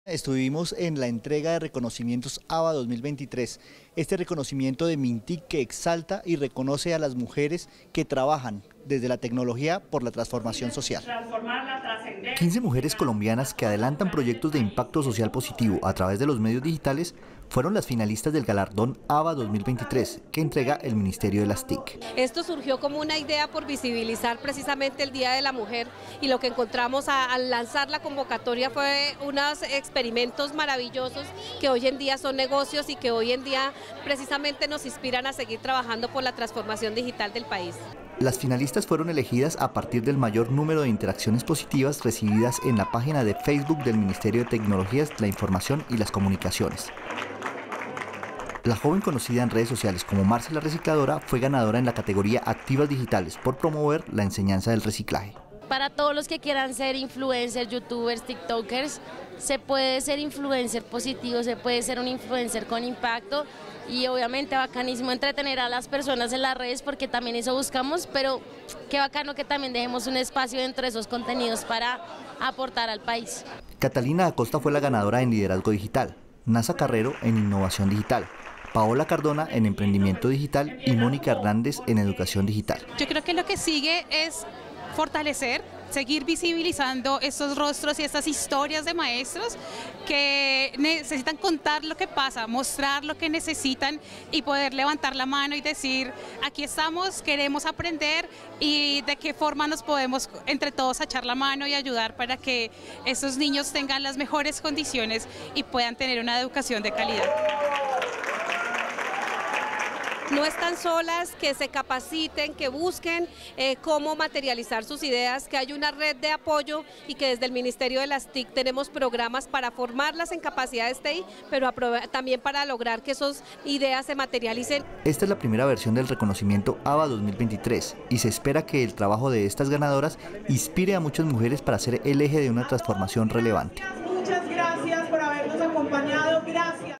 MinTIC reconoce proyectos digitales de impacto social liderados por mujeres | Noticias Enlace Trece
🟠📺 EnlaceTrece | ¡Las mujeres que lideran proyectos transformadores a través de las herramientas digitales son un ejemplo a seguir! ¡No subestimemos el poder de un like para apoyar! 💪👩‍💻